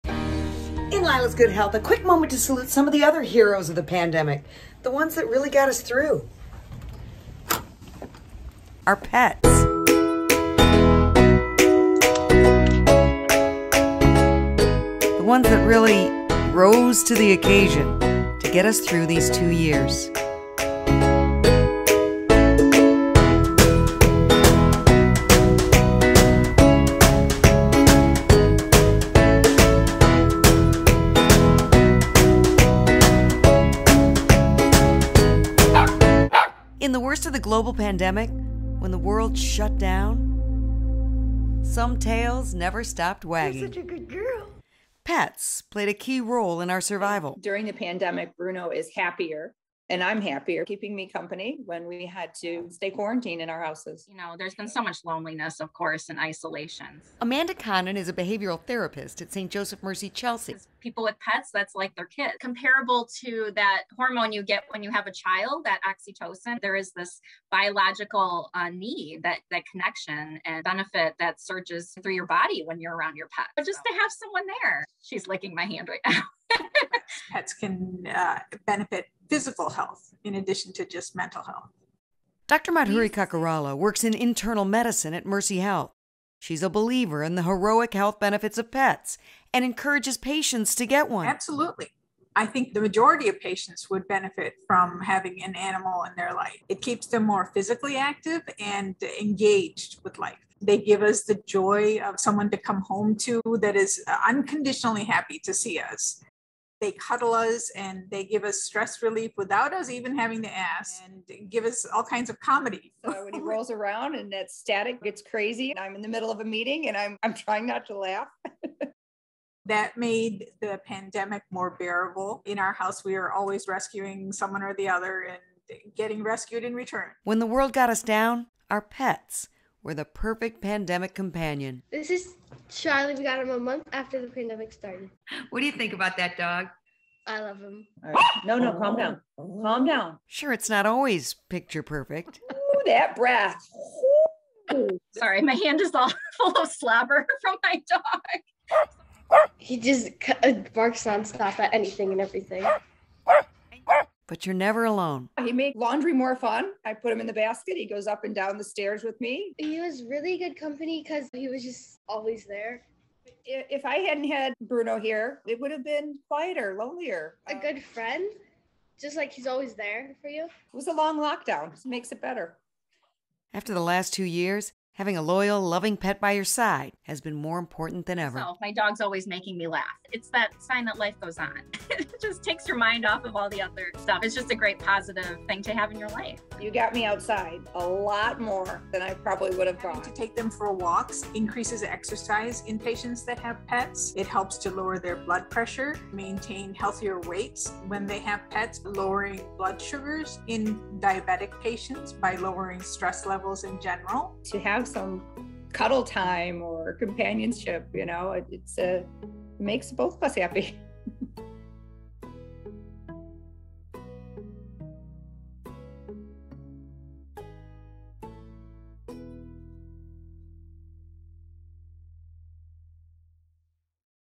The HEALING POWER OF PETS | Lila's Good Health
In addition to those on the front line, there was another powerful bunch who helped many of us get through the last two years.  Health Reporter Lila Lazarus tells the 'tail' of our 4-legged heroes – the dogs, cats and even birds that helped us persevere.  If you’re looking for a surefire way to lose weight, combat loneliness, feel happier and less isolated, this could be the answer.  And yes, doctors are recommending it.